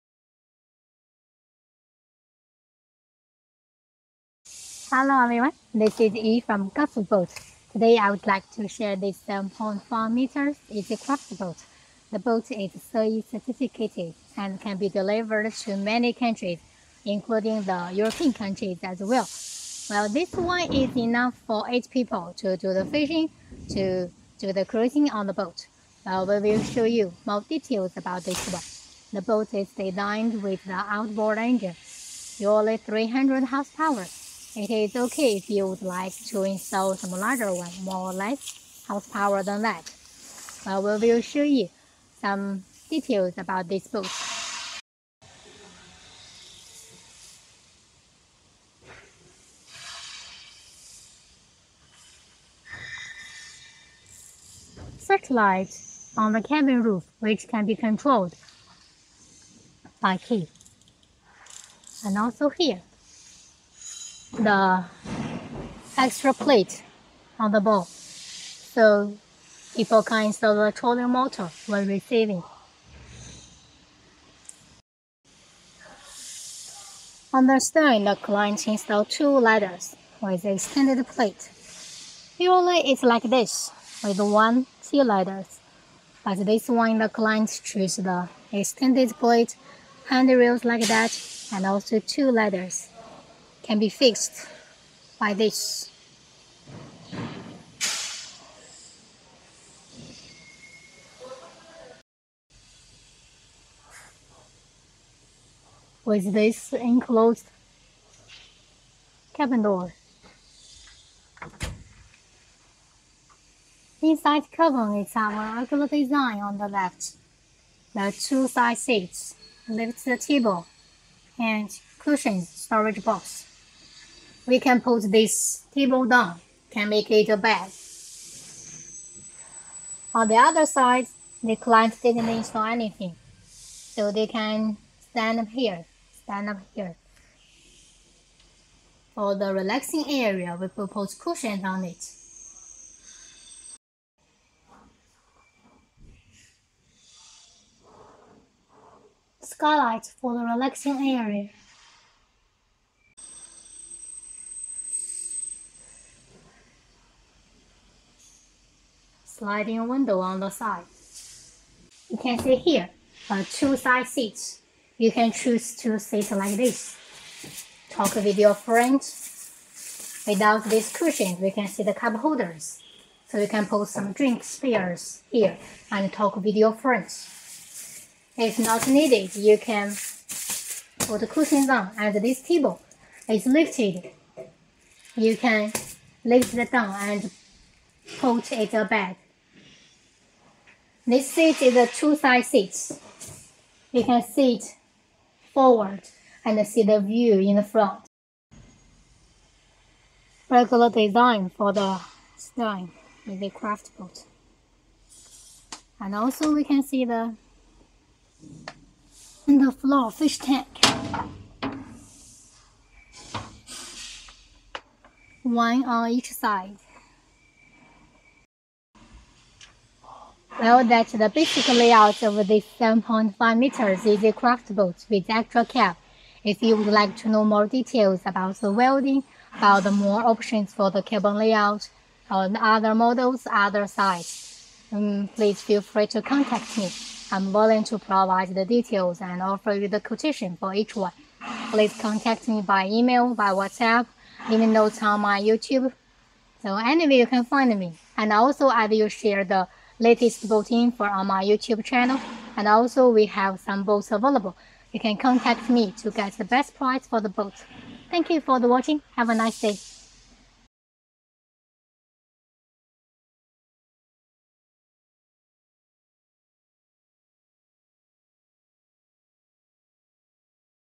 Gospel Boat Easy Craft 7.5m Easy Craft Extra Cabin Aluminum Fishing Boat for Sale
This is 7.5m x 2.45m Easy Craft Aluminum Fishing Boat
Enclosed Cabin Door; Extra Cabin; Lifted Table

I am willing to provide more details and latest info